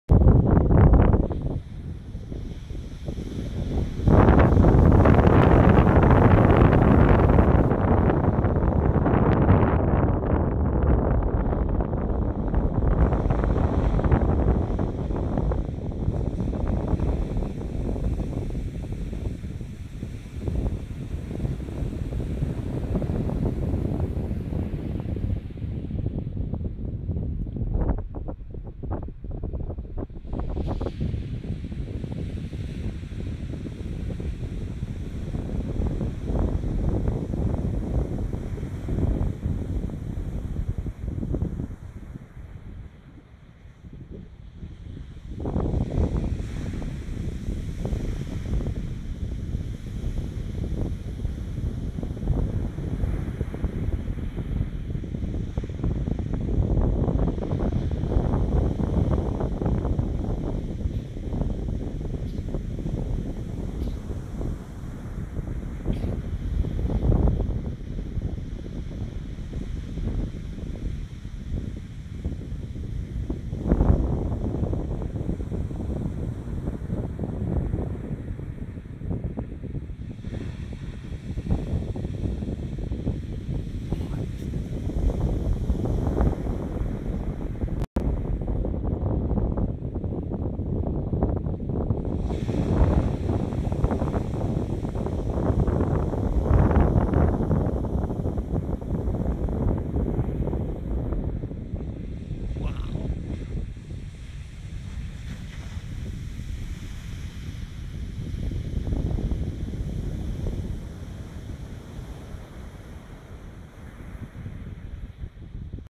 Huge waves on the North Shore of Oahu
December on the North Shore, near Pipeline.